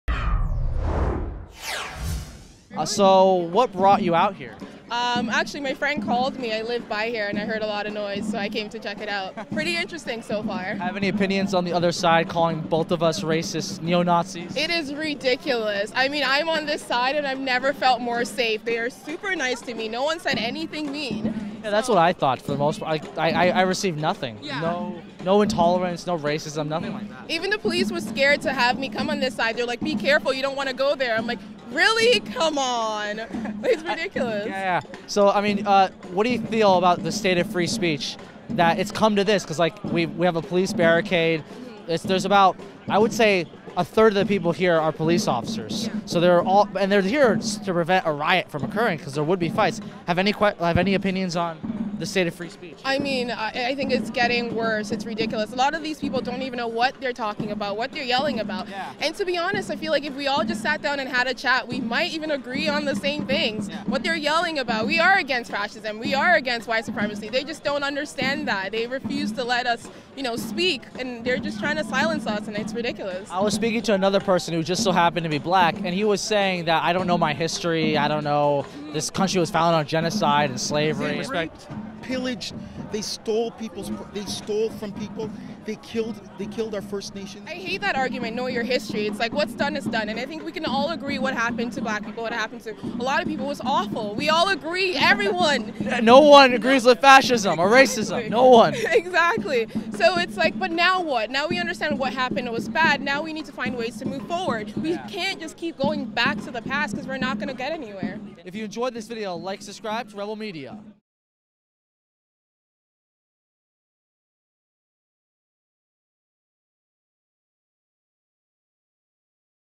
Student free speech activist: We're NOT fascist or racist!
Jay Fayza of TheRebel.media attended a rally against M-103 where he spoke with student free speech activists Josephine Mathias. They discussed the state of free speech, stereotypes, and moving forward. MORE